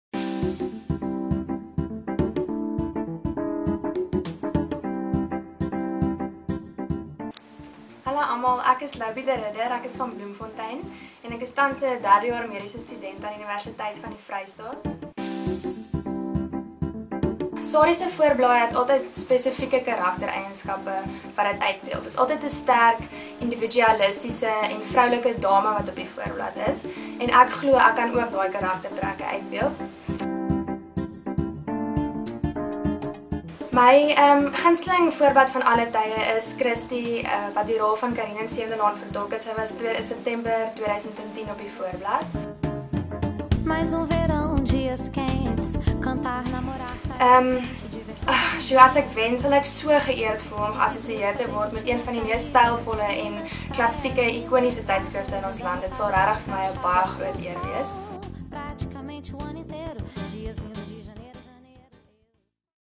SARIE-voorbladgesigkompetisie - Loubè de Ridder.mp4
SARIE-vroue het dit! Na weke se kopkrap en keuring het ons SA (en Namibië) se 35 mooiste voorbladgesigte uit meer as 3 000 inskrywings. Hier is Loubè de Ridder, een van die 35 finaliste, se videos.